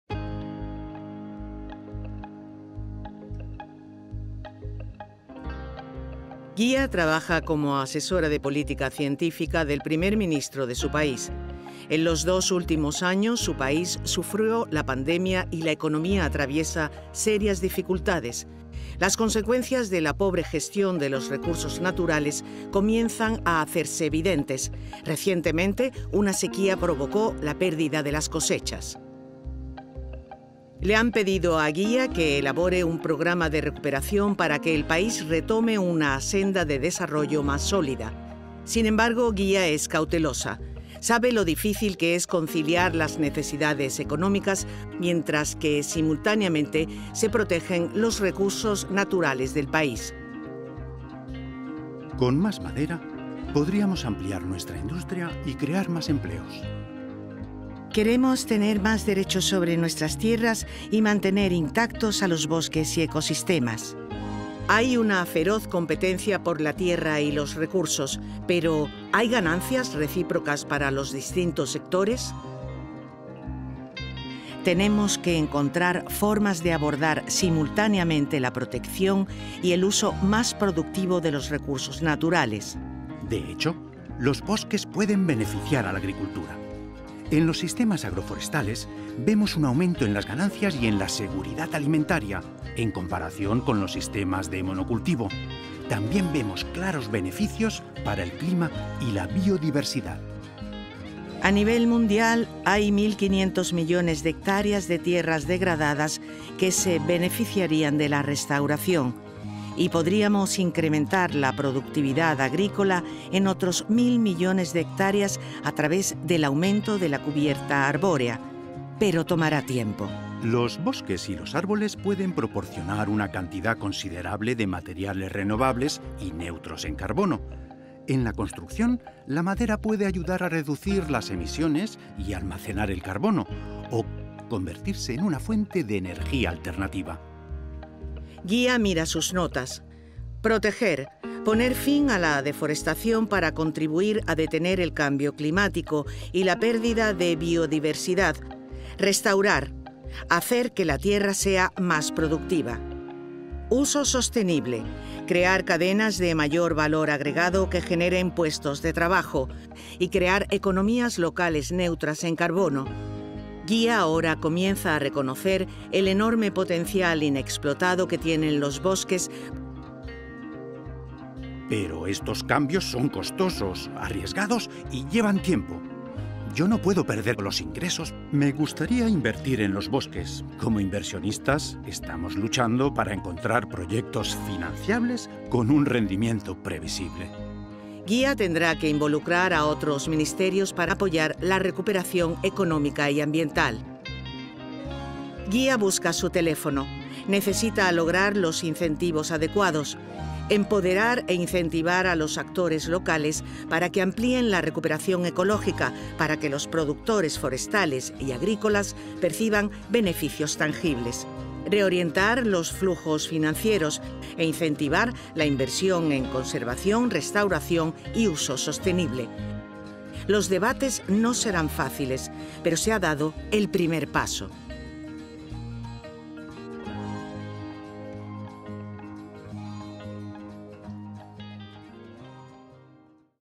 Geya y las tres trayectorias.
En esta animación gráfica, el personaje ficticio de Geya, que trabaja como asesora científica del Primer Ministro de su país, reflexiona sobre la viabilidad de implementar soluciones forestales (conservar  – restaurar – utilizar de manera sostenbile) como base para recuperación verde.
Sin embargo, Geya es cautelosa. Sabe lo difícil que es conciliar las necesidades económicas para generar ingresos y medios de vida, brindar un suministro continuo de materiales y apoyar las necesidades de seguridad alimentaria, mientras que simultáneamente se protegen los recursos naturales del país.